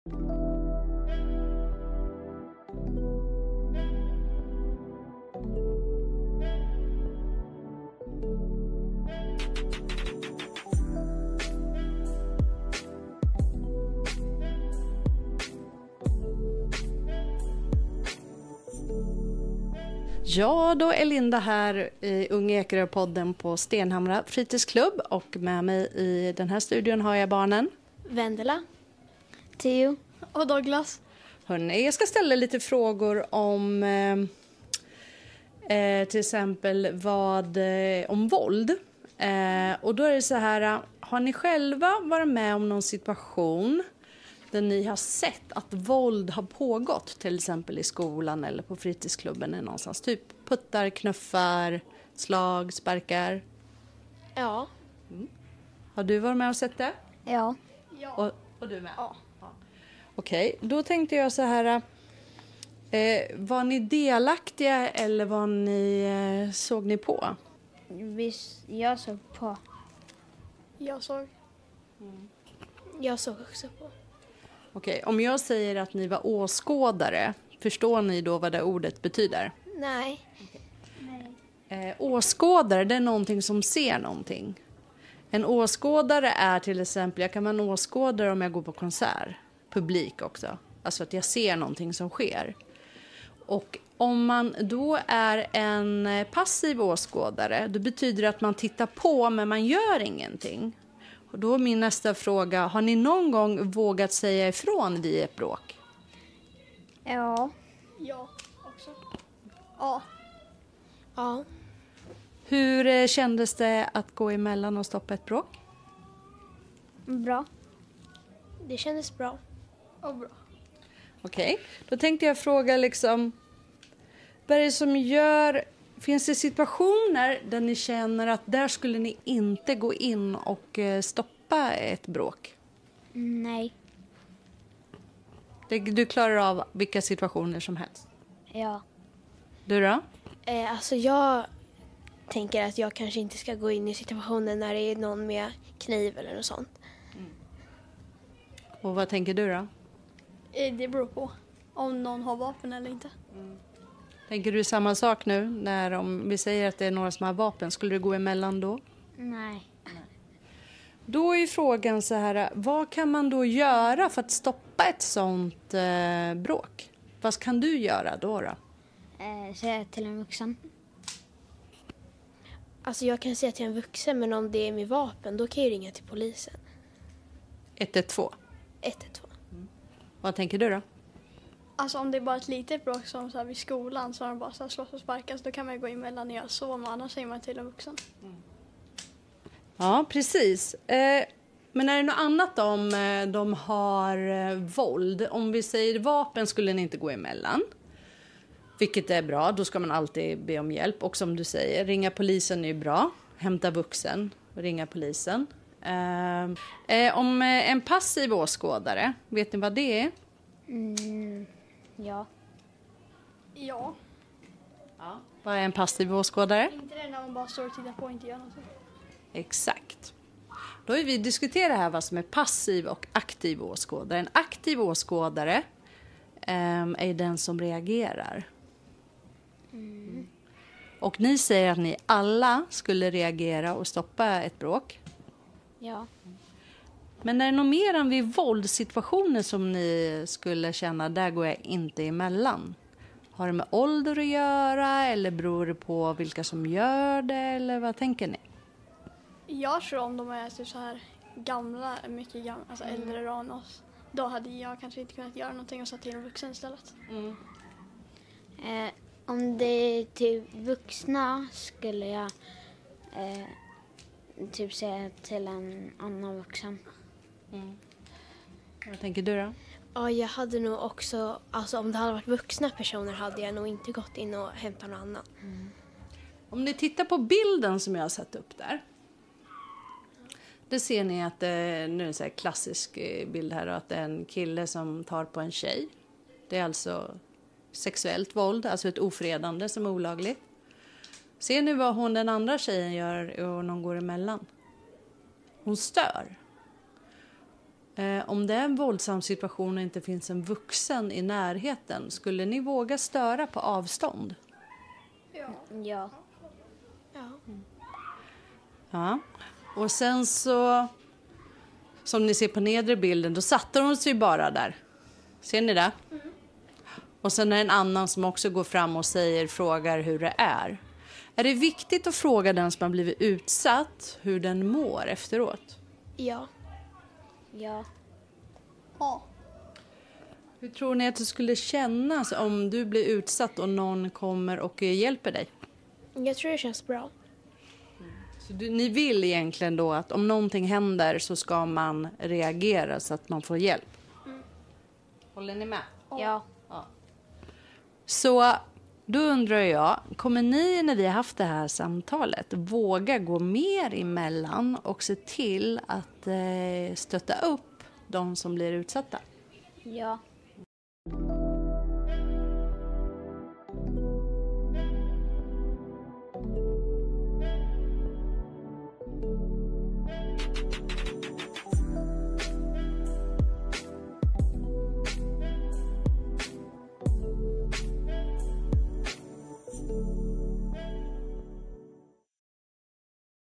Modiga barn som vågar agera vid bråk
Här kan ni lyssna på en intervju vi haft med fritidsklubbens barn på Stengården i stenhamra. De får svara på frågor om våldssituationer och om när de skulle agerar och försöka stoppa det eller kalla på hjälp.

Varför vi intervjuar barn just nu i dessa frågor är för att vi i Ung i Ekerö just nu jobbar med MVP -mentorer i vålds prevention. Vi har under arbetet upptäckt att varje elev i årskurs 4-6 bevittnar våld minst en gång i veckan, oftast på ställen där vuxna inte finns.